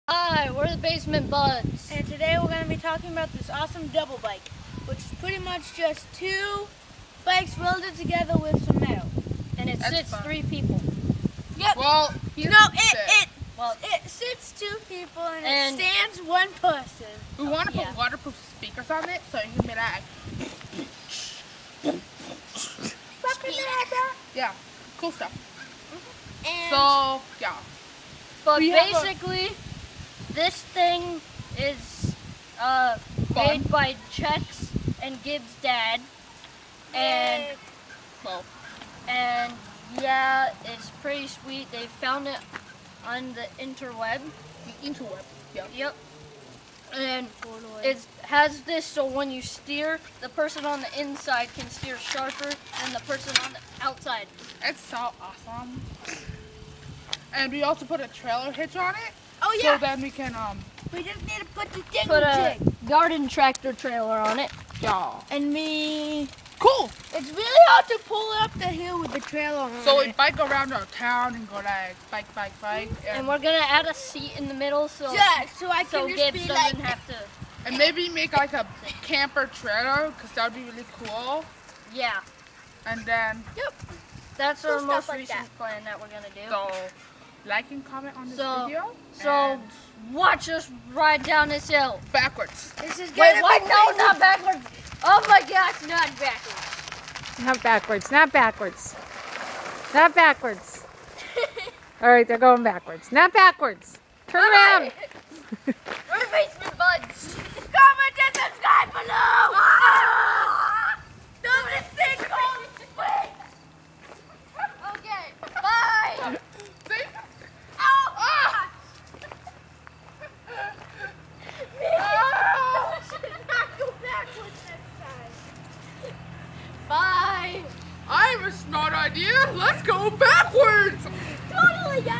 Introducing The Double Bike!
This awesome double bike is to bikes welded together into an awesome bike that you can  ride on with your friend! You can even put a hitch on it and pull a trailer.